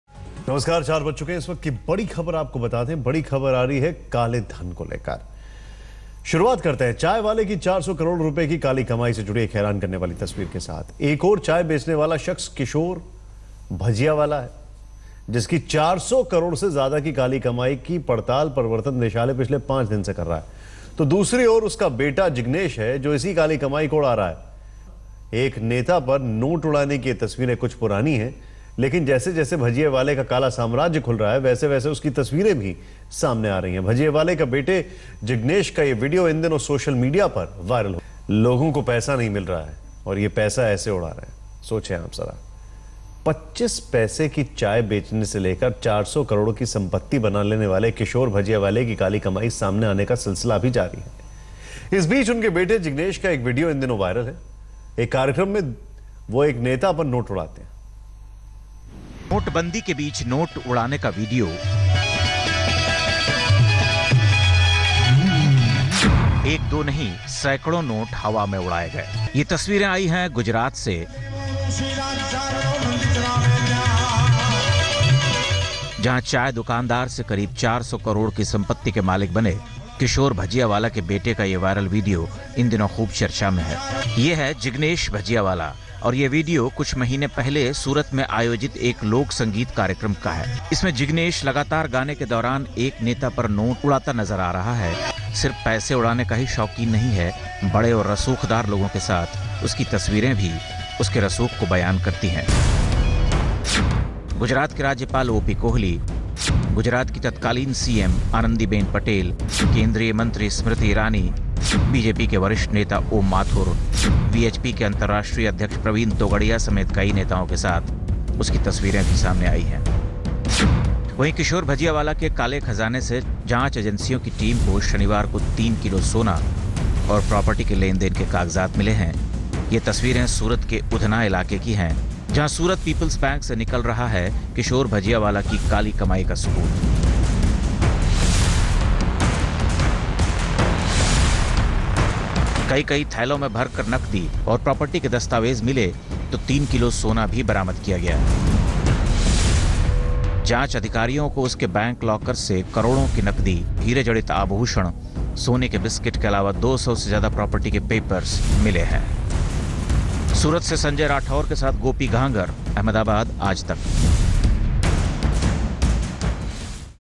Jignesh Bhajiyawala Viral Video
The Income Tax department on Friday uncovered assets worth Rs 150 crore and cash in raids conducted at the house and offices of Kishore Bhajiyawala, the Surat- based teaseller-turned-financier.